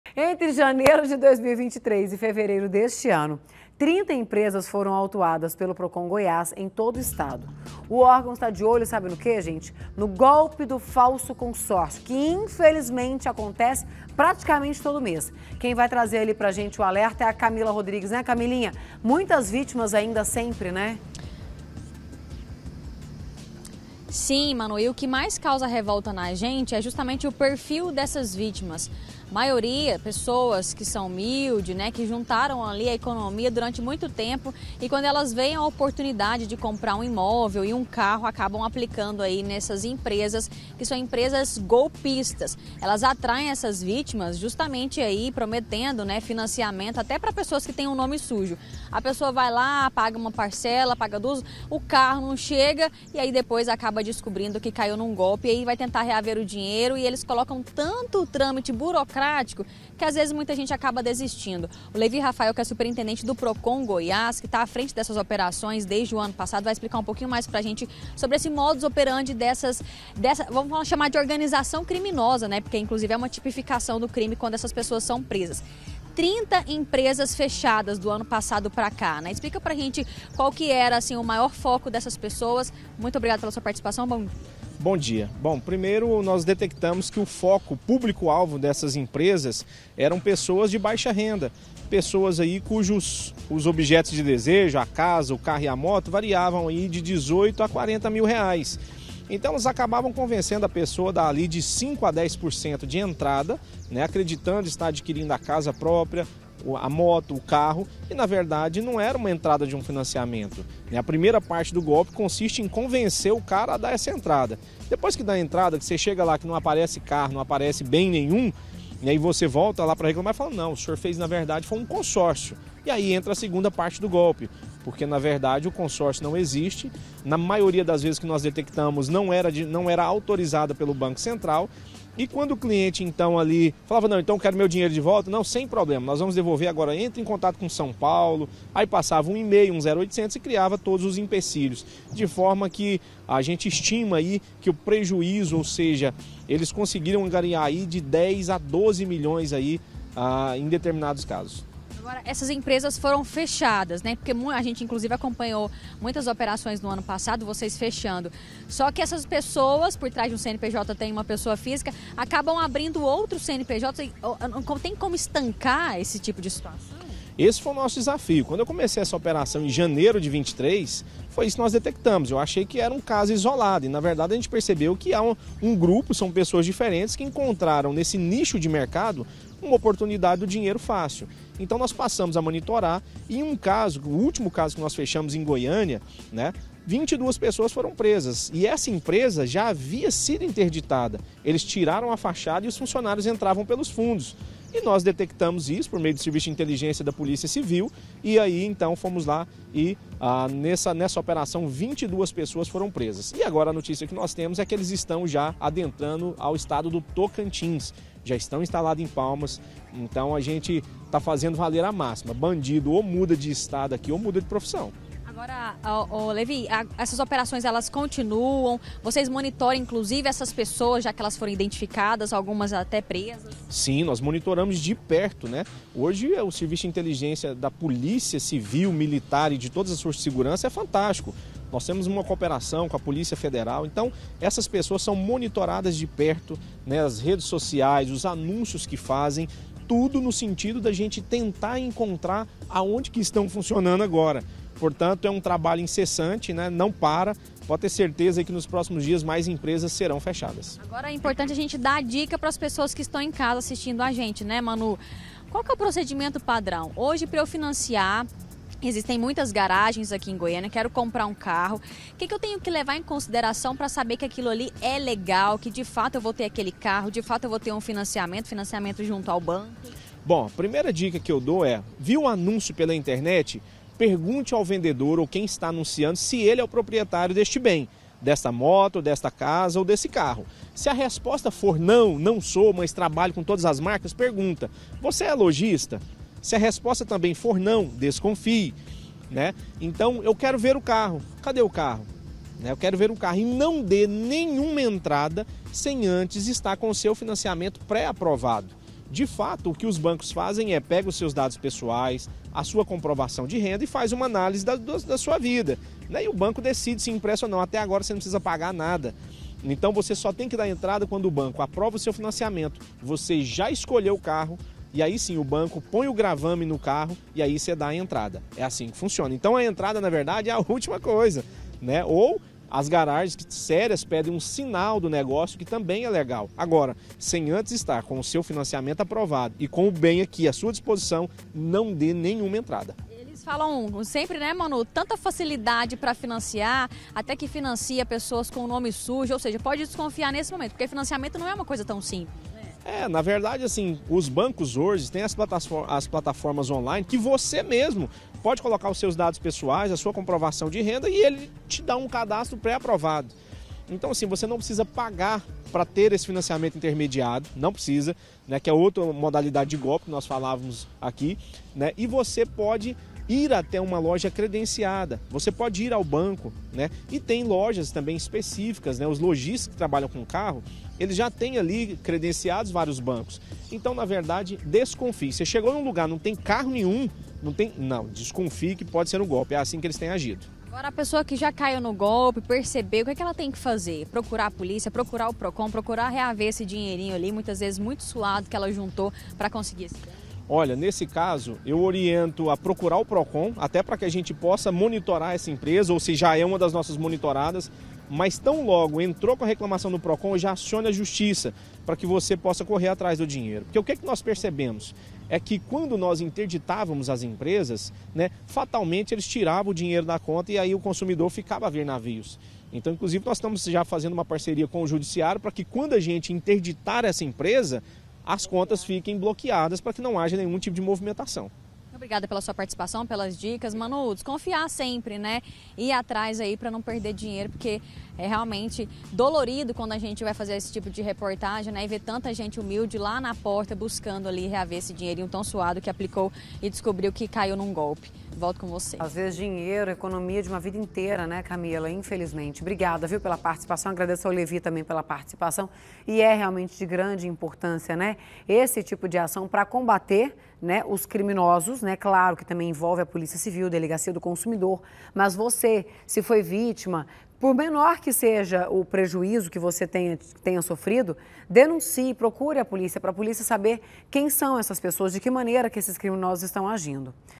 FALSO CONSÓRCIO: PROCON ATUA CONTRA GOLPISTAS
ENTRE JANEIRO DE 2023 E FEVEREIRO DESTE ANO, 30 EMPRESAS FORAM AUTUADAS PELO PROCON GOIÁS EM TODO O ESTADO. E O ÓRGÃO ESTÁ DE OLHO NO GOLPE DOP FALSO CONSORCIO.